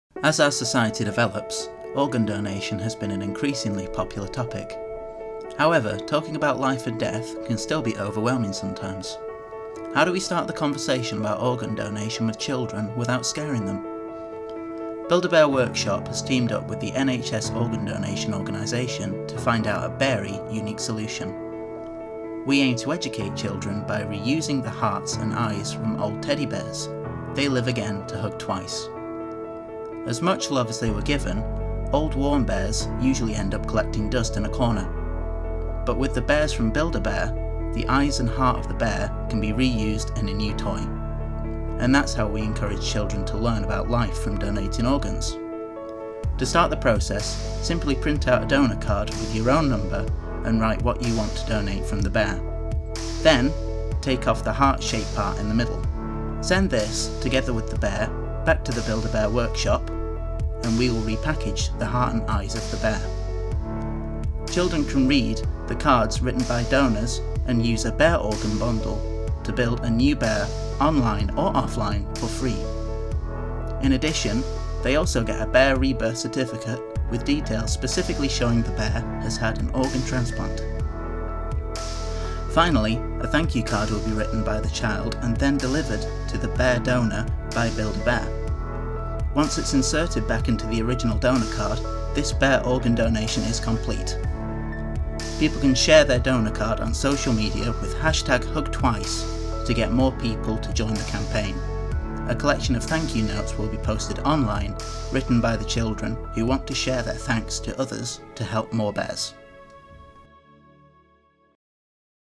Hug Twice
Work by 2nd Year Creative Advertising students Ran and Poppy in response to the brief ‘Take something of value and give it value’. In this case how do we use old teddy bears to start the delicate discussion about organ donation with young children?